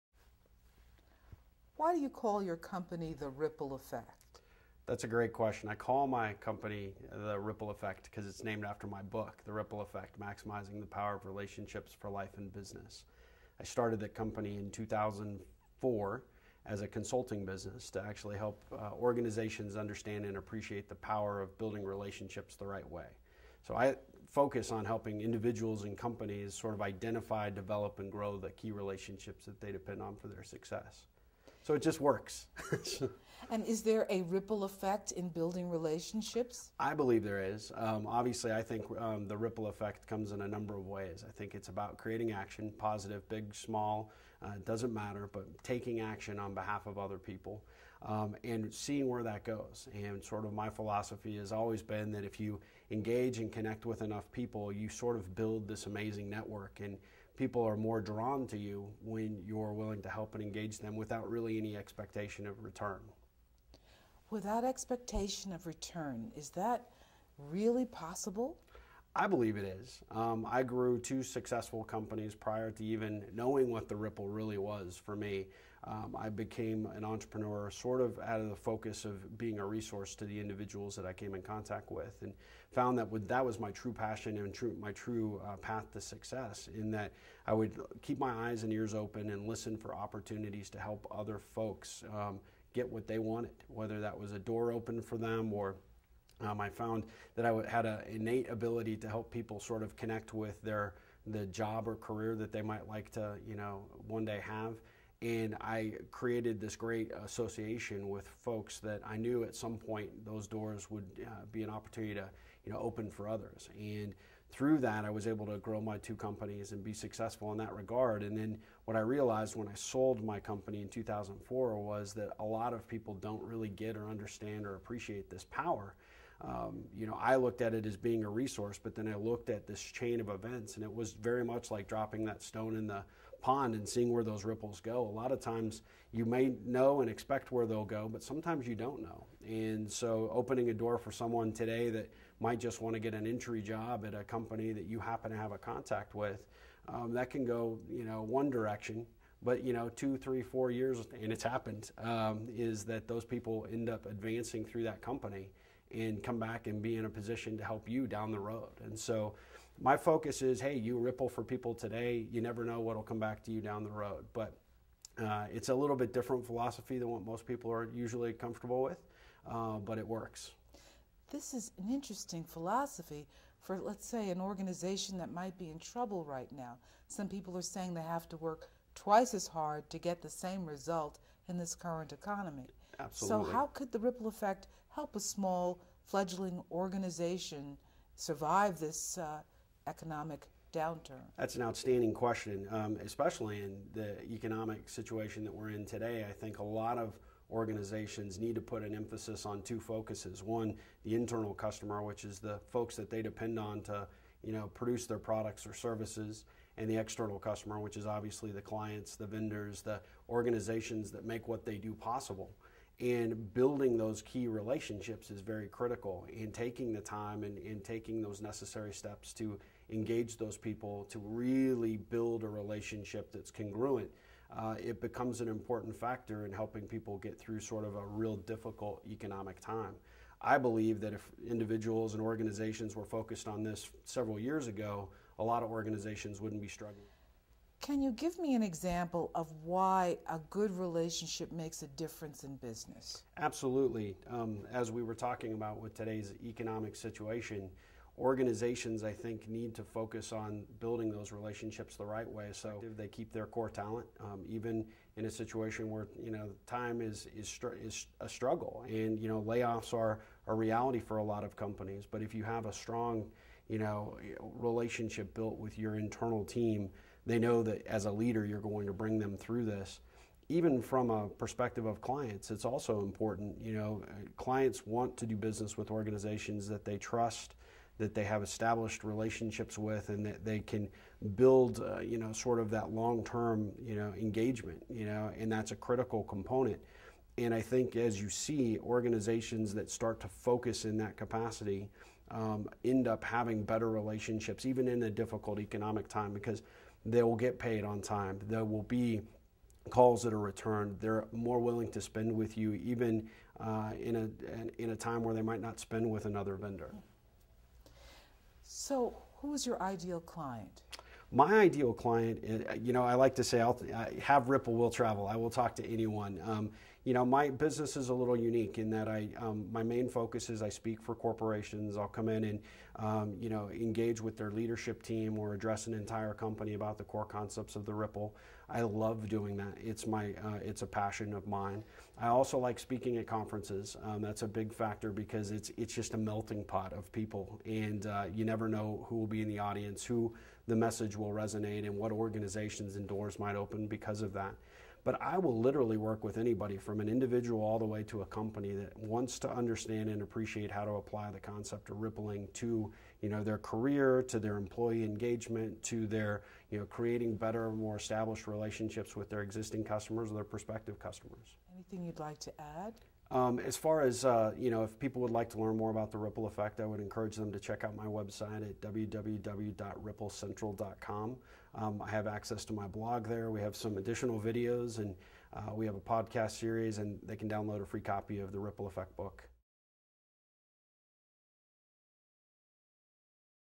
Steve Harper
Steve Harper author of  The Ripple Effect: Maximizing Relationships in Business and in Life is interviewed by producer Phyllis Haynes.  Cameraman is Robbie
Anderson.